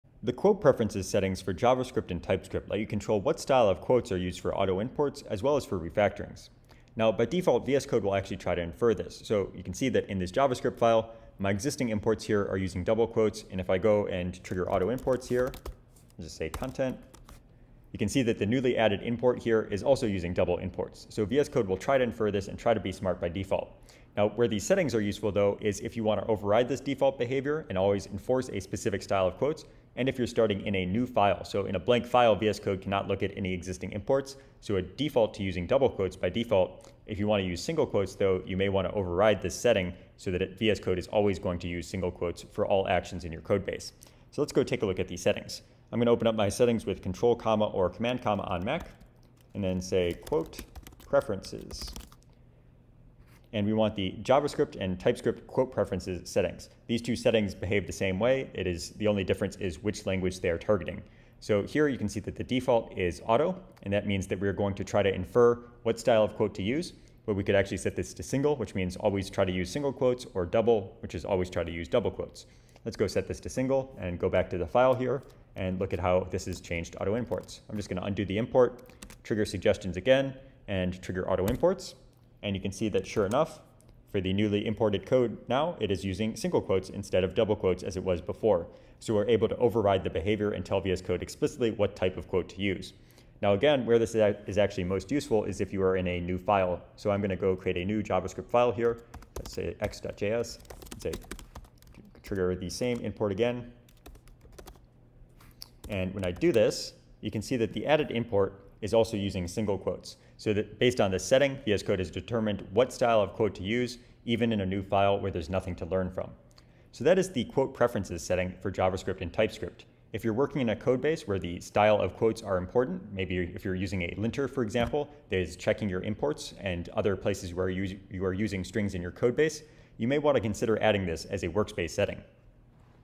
VS Code tips — Changing the prefered quote style in JavaScript and TypeScript
Today's VS Code setting: javascript.preferences.quoteStyle

Tell VS Code to always use single or double quotes for auto imports and refactorings.

This is useful for new files where VS Code cannot infer which style of quotes to use from your existing code.

For typescript, use typescript.preferences.quoteStyle